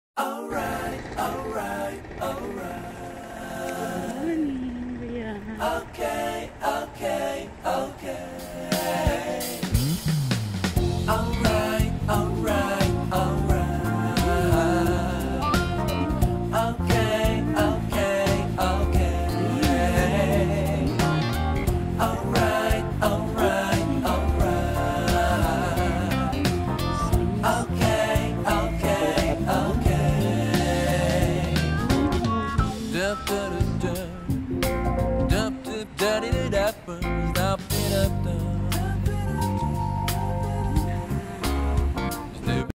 OFF from work. After 4 nights duty 3 days off yeheeey.
Yong pag pauwi kana from duty at mag OFF kana grabe na ang saya mo.